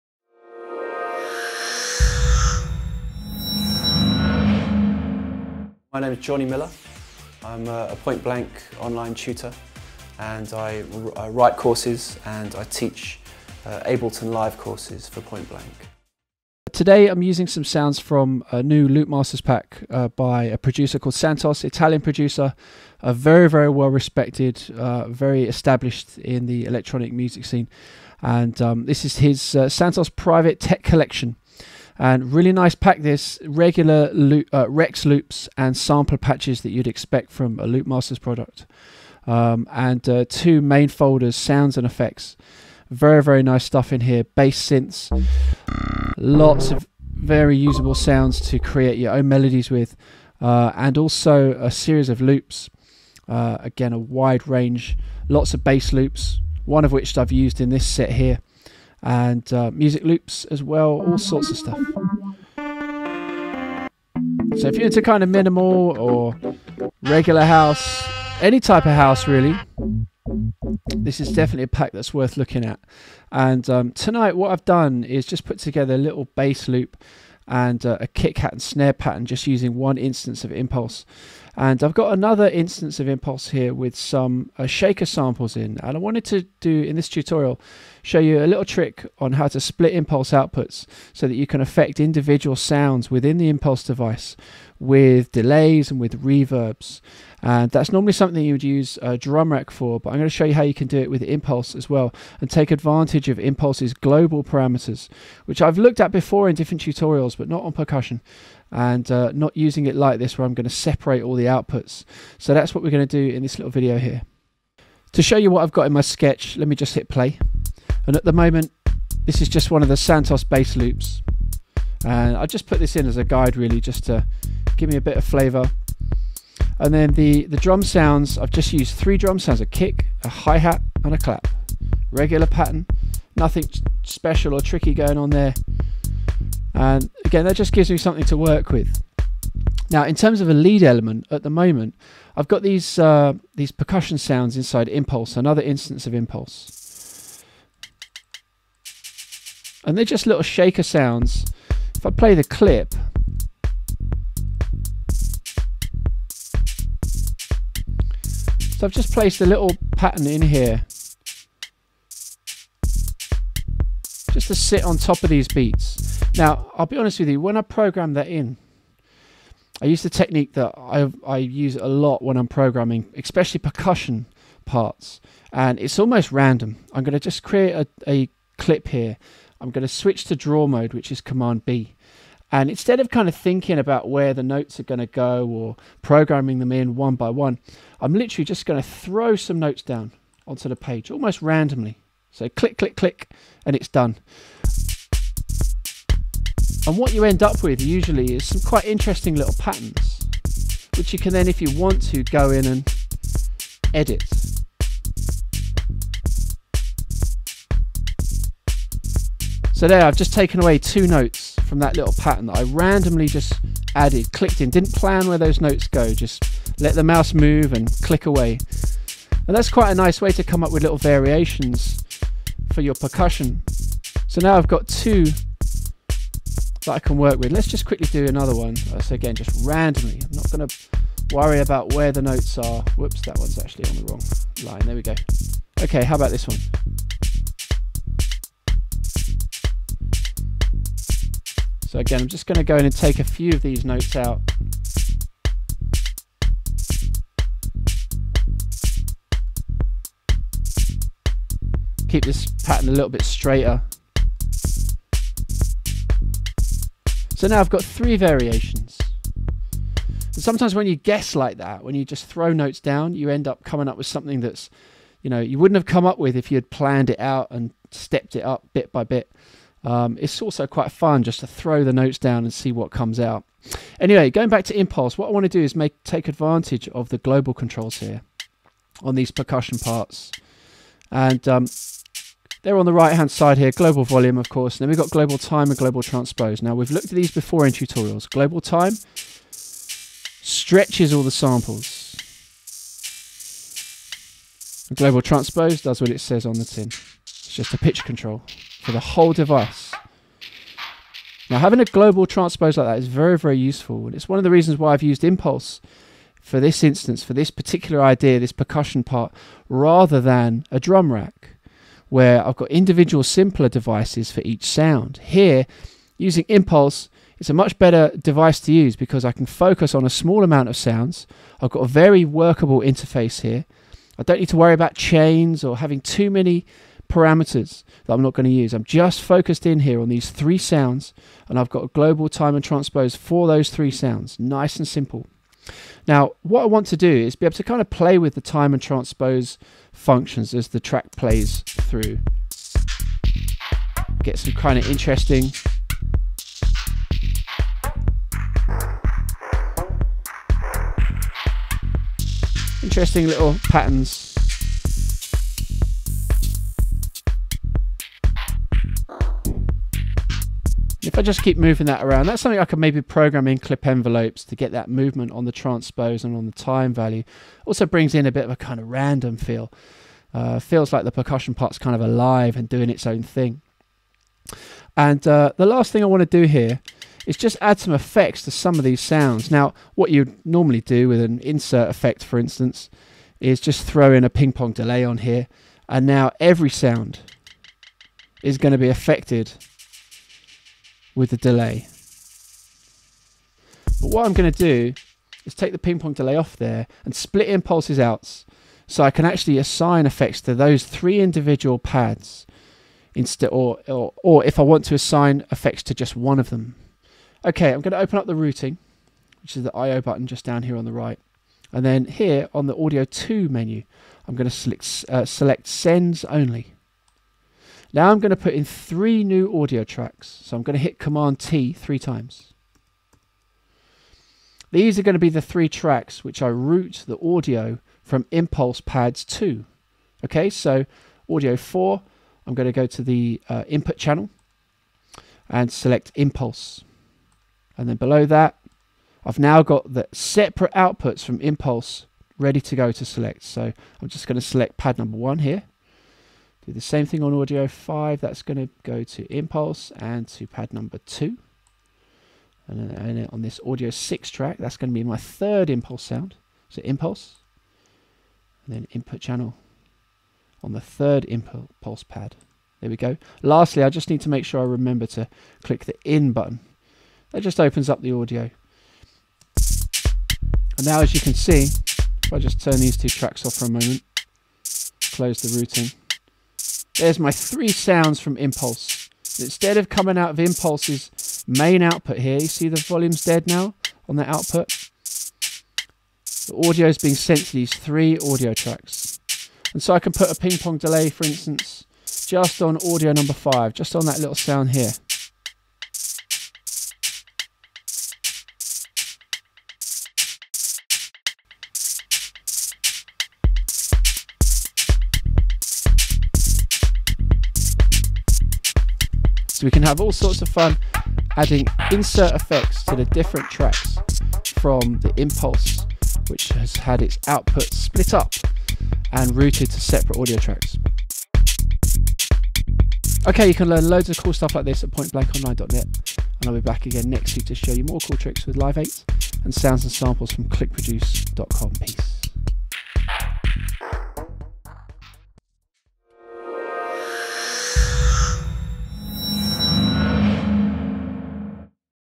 Ableton Live Tutorial - Impulse - Split Outputs to Bring Percussion to Life
In this video Jonny Miller (Jus'Listen/Sonarpilot Audio) demonstrates how to split the outputs within impulse so you can effect individual percussion sounds within the device with delays and reverbs.

He is using samples and loops from the Santos: Private Tech Pack